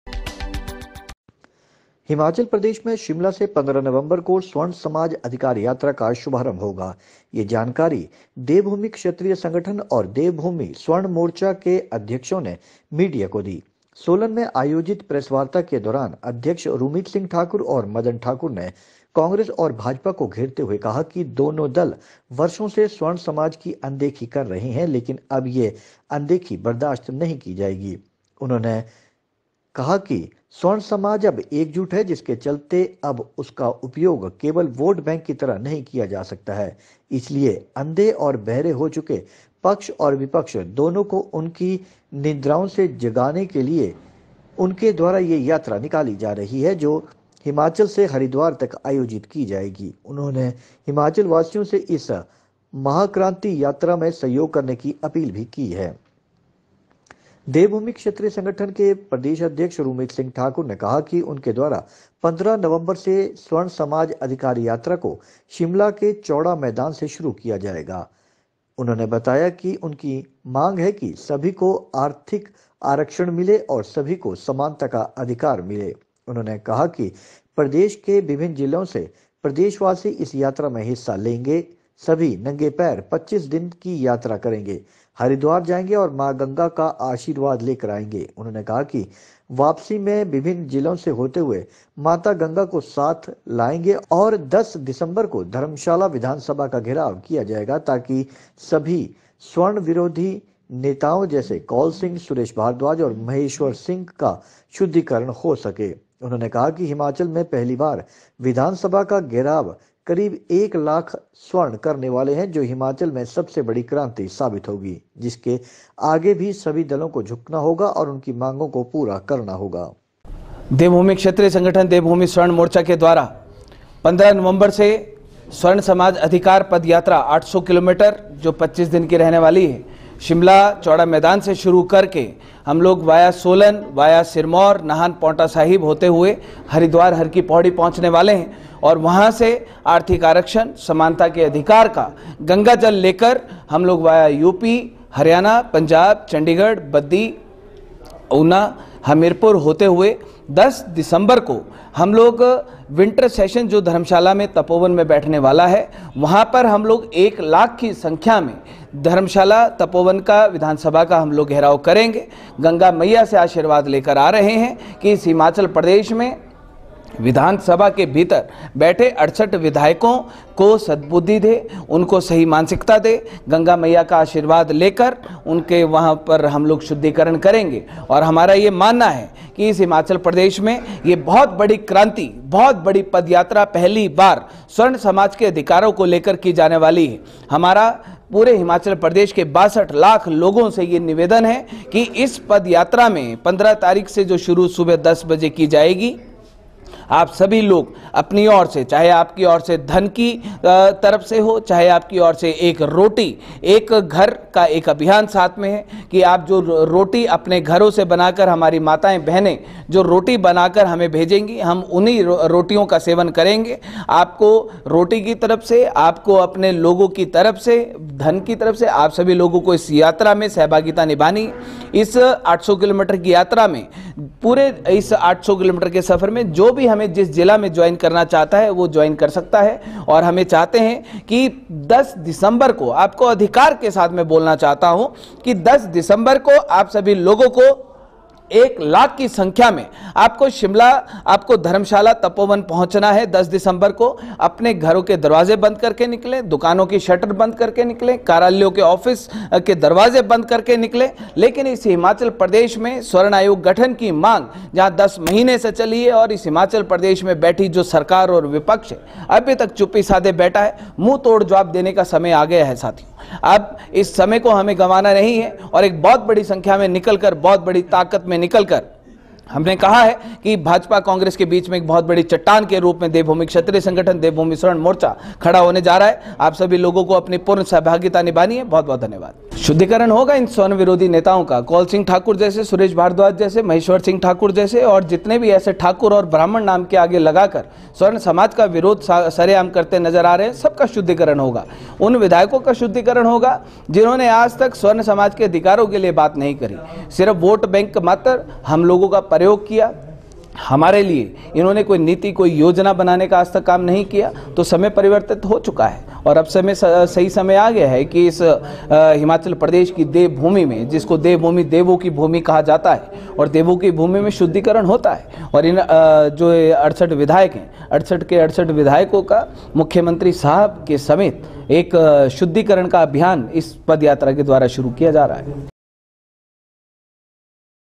solanनेताओं की शुद्धिकरण केलिए गंगा मैया को लेकर नंगे पैर आएँगे,करेंगे विधानसभा का घेराव:रुमीत सिंह
हिमाचल में  शिमला से. 15 नवंबर को  आरम्भ होगी, सवर्ण समाज  अधिकार यात्रा। यह जानकारी देवभूमि क्षत्रिय संगठन। और देव भूमि सवर्ण मोर्चा के अध्यक्षों ने, मीडिया को दी।  सोलन में आयोजित प्रेस वार्ता के दौरान, अध्यक्ष रुमीत सिंह ठाकुर,और  मदन ठाकुर ने ,कांग्रेस और भाजपा को घेरते हुए कहा कि ,दोनों दल वर्षों से   स्वर्ण समाज की ,अनदेखी कर रहे हैं। लेकिन अब यह अनदेखी ,बर्दाश्त नहीं की जाएगी।  उन्हें बताया जाएगा कि ,स्वर्ण समाज अब एकजुट है , जिसके चलते अब उनका उपयोग, केवल वोट बैंक की तरह  नहीं,  किया जा सकता है।  इस लिए  अंधे और बहरे हो ,चुके पक्ष और विपक्ष दोनों  को  ,उनकी निंद्रा से जगाने के लिए ,उनके द्वारा यह यात्रा, निकाली जा रही है।  जो हिमाचल से हरिद्वार तक ,आयोजित की जाएगी। उन्होंने हिमाचल वासियों से, इस महा क्रांति यात्रा में ,सहयोग की अपील भी की है।